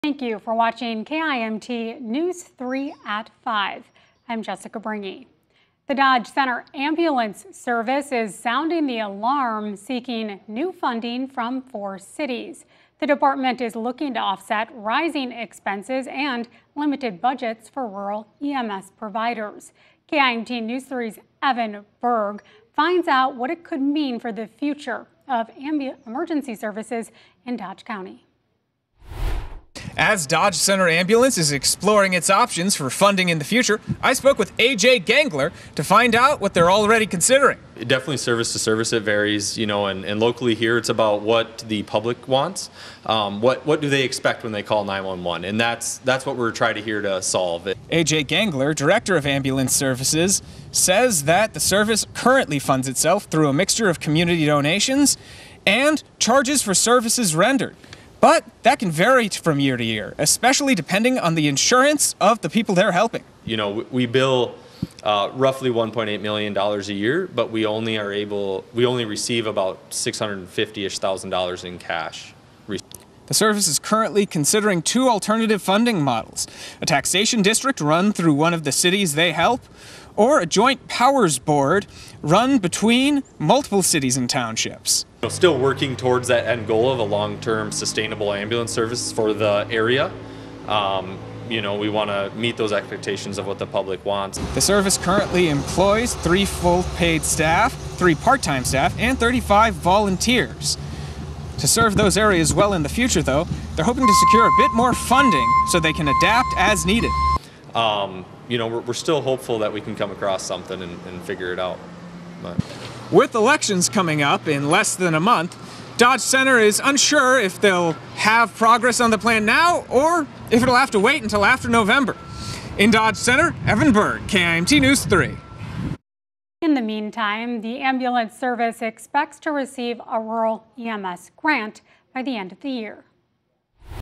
Dodge Center Ambulance Service considers new funding options
KIMT News Three's Evan Berg spoke with the director on funding woes and what the department is currently considering.